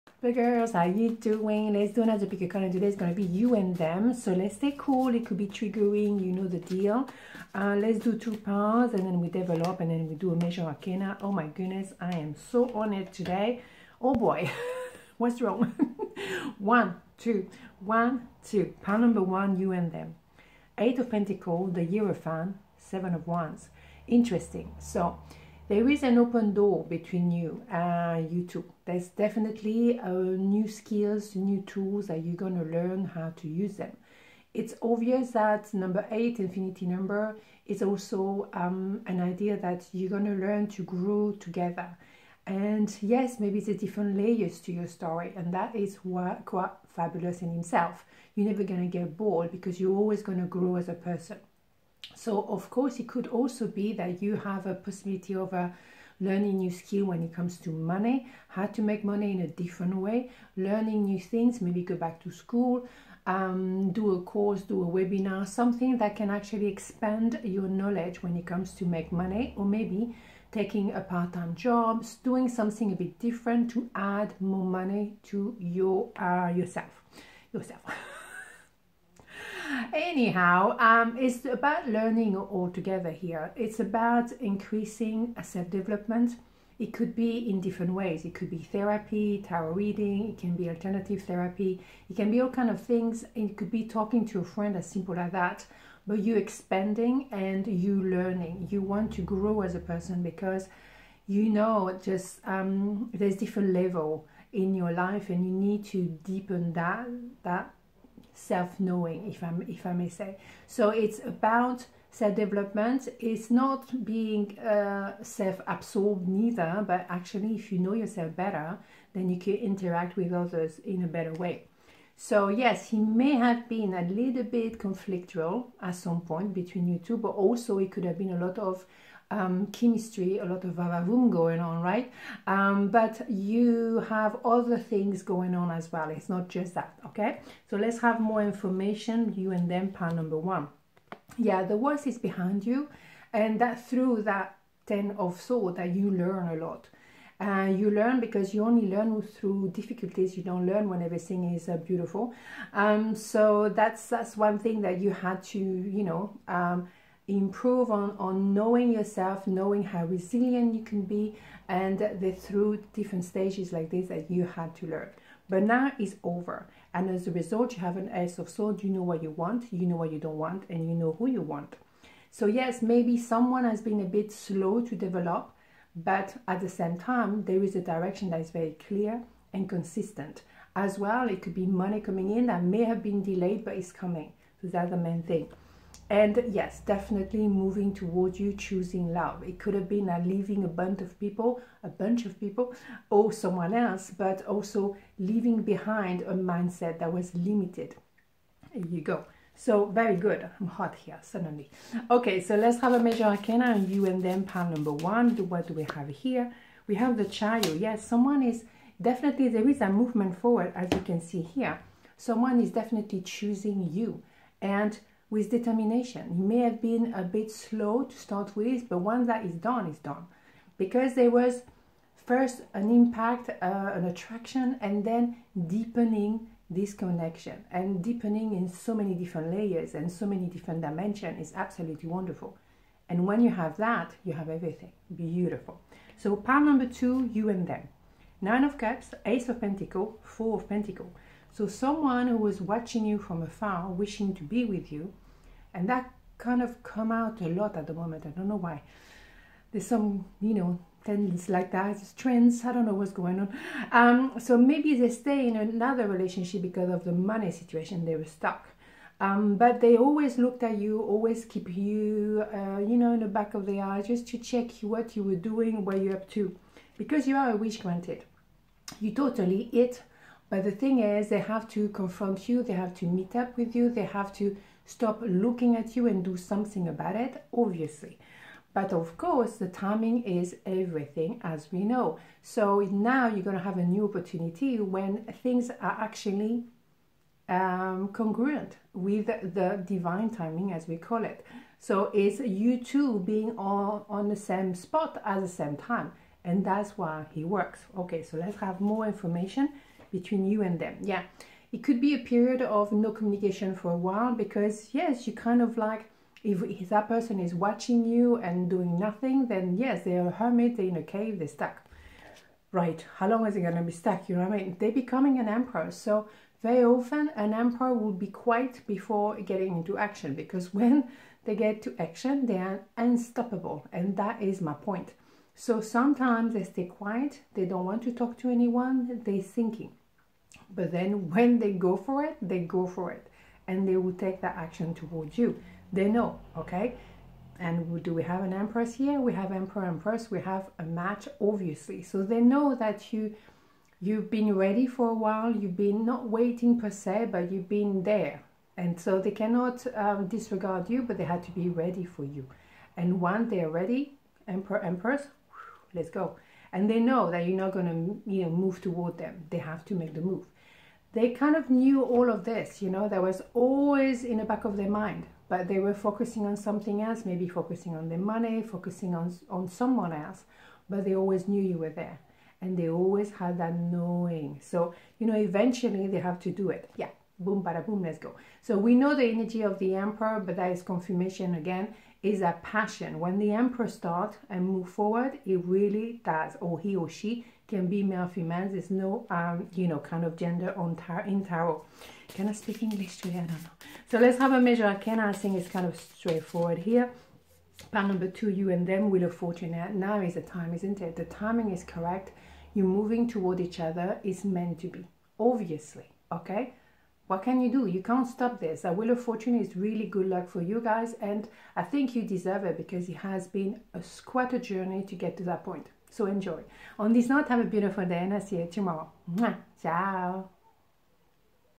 Pick a card: you and them
Hi this is Axelle, welcome to another Pick a card tarot reading!
Today is all about you and them.
Pile 1: 0:19   Pile 2: 3:56

Hopefully this will put a smile on your face, and will bring some clarity to whatever your situation is. As you know, this is a general reading therefore it might not resonate with everyone. Follow your intuition, take a breath and choose a pile. Sometimes both piles can be of interest so do not hesitate to watch both. Also some subjects can be triggering so please, take them with a pinch of salt as tarot is a tool, a different approach and perspective on things. Et voila!

*Decks used:
- Juliet Sharman-Burke
- K. Fredheim, C. Nika, E. Ekelund for Soul cards tarot

Entertainment purposes only. Thank you.